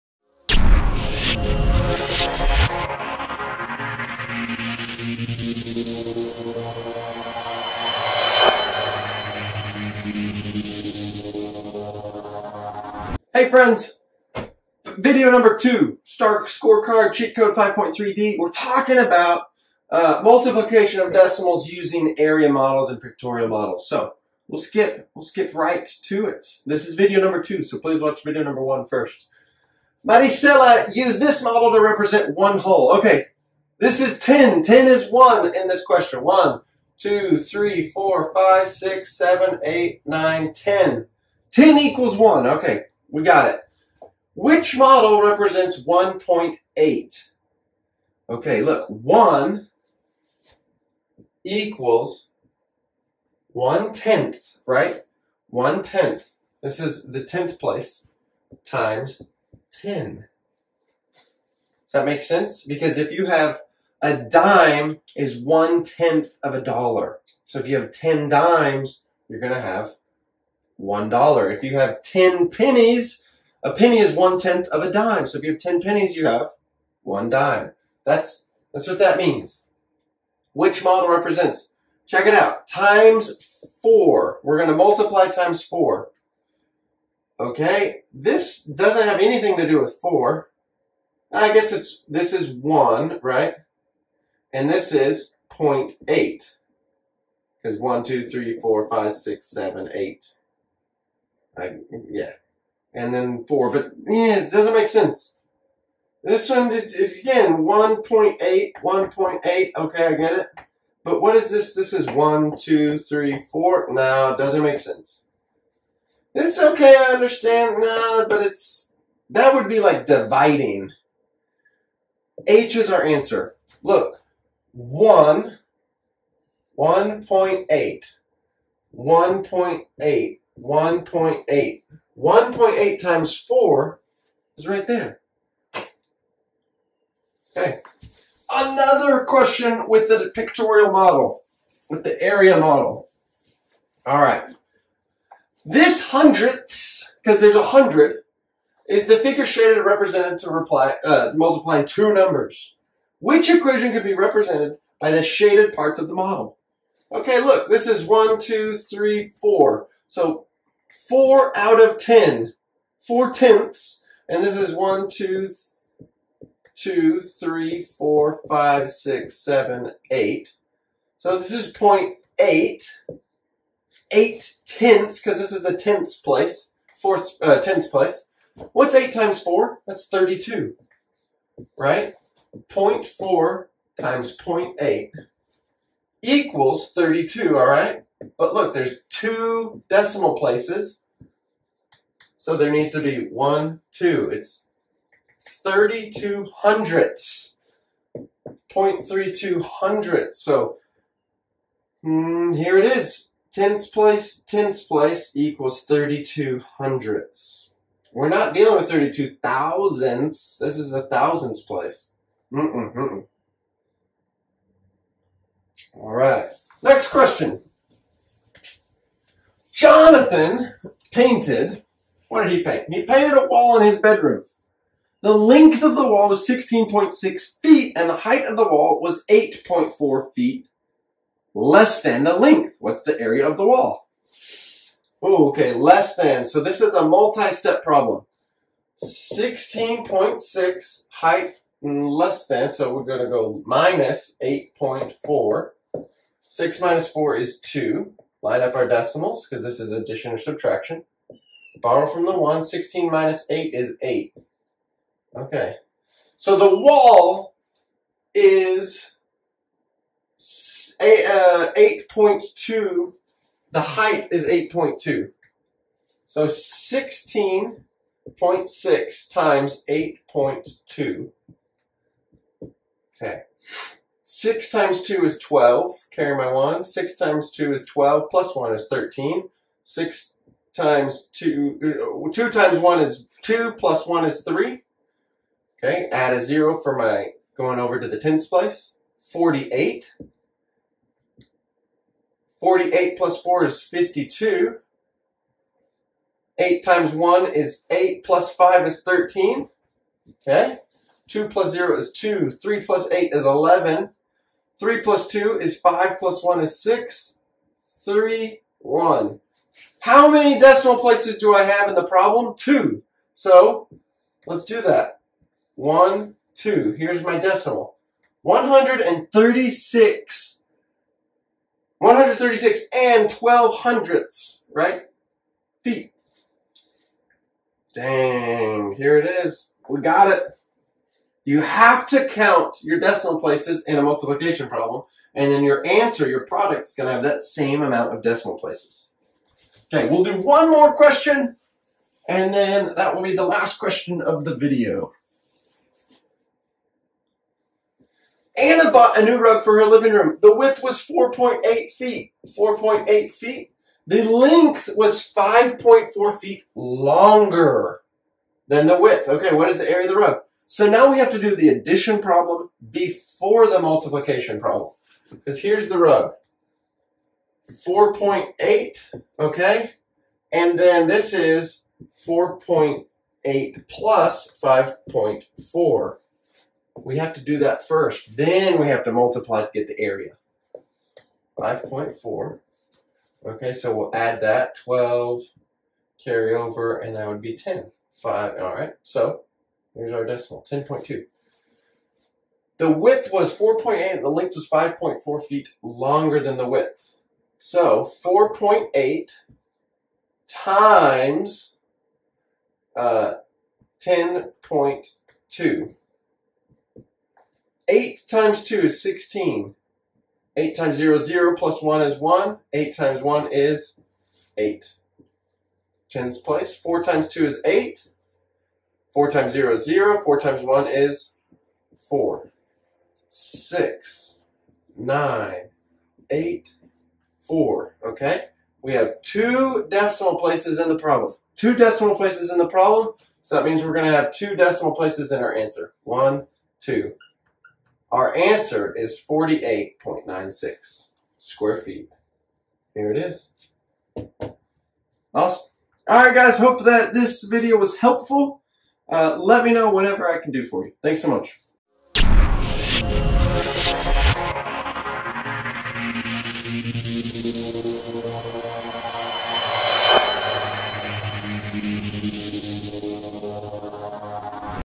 5.3(D) #2 - Multiply decimals with models
5.3(D): Represent multiplication of decimals with products to the hundredths using objects and pictorial models, including area models.